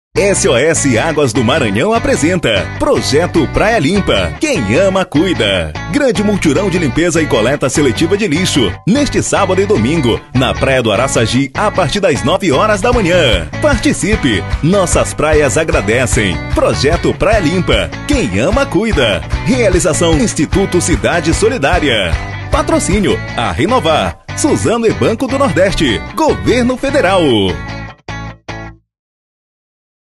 Participe do Projeto Praia Limpa!
Seja voluntário e participe da 2ª edição do Projeto Praia Limpa. Uma iniciativa que visa a coleta de lixo e campanha de conscientização com banhistas, comerciantes e frequentadores para a necessidade de cuidar do nosso meio ambiente. Venha conosco!